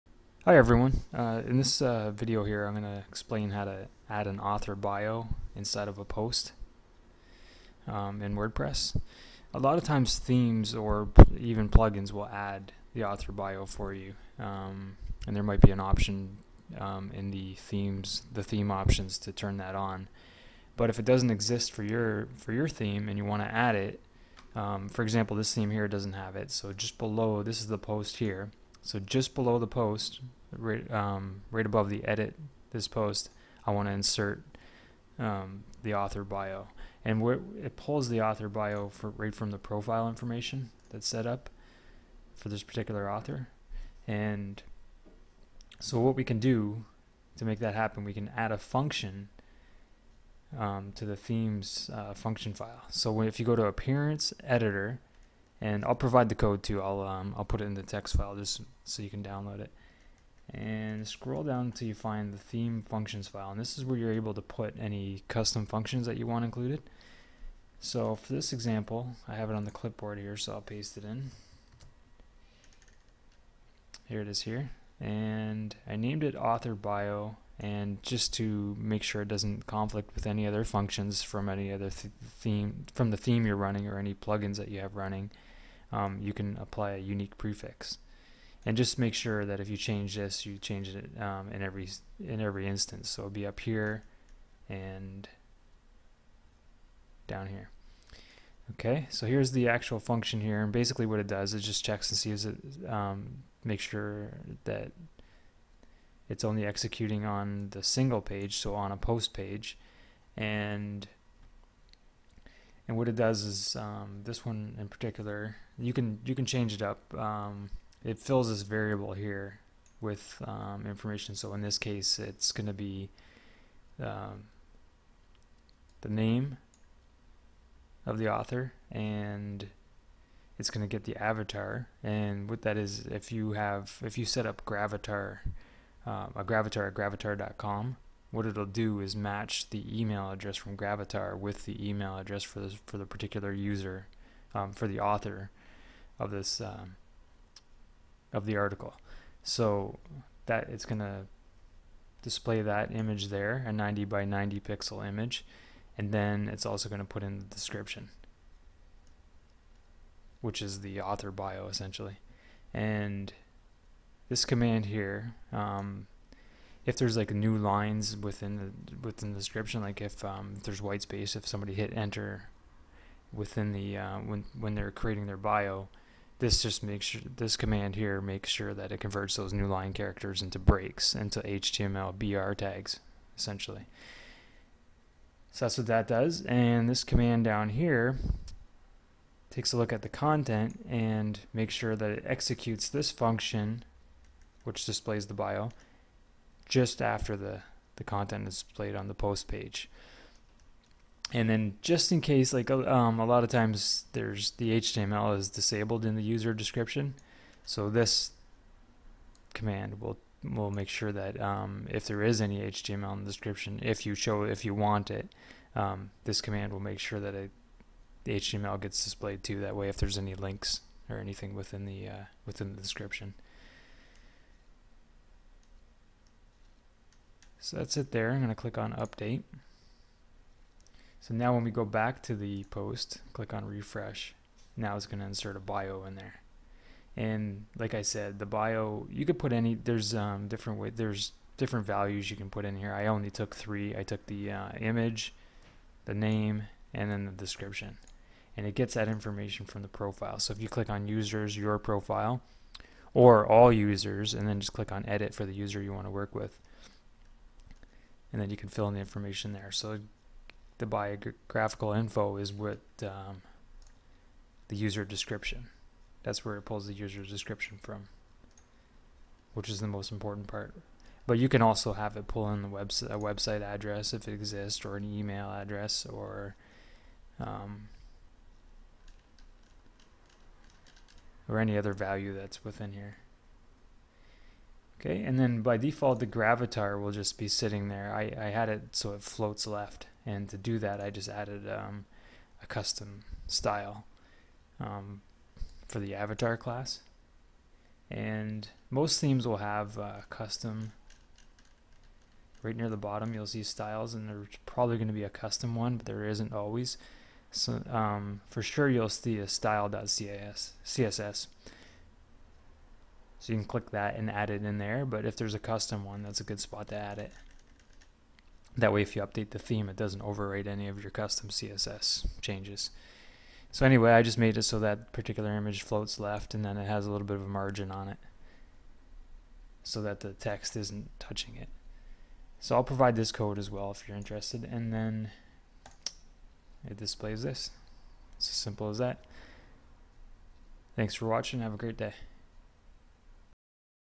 Adding an Author Bio Box in WordPress
This video tutorial will show you how to add an author image and bio box in WordPress, and Link to a Google+ Profile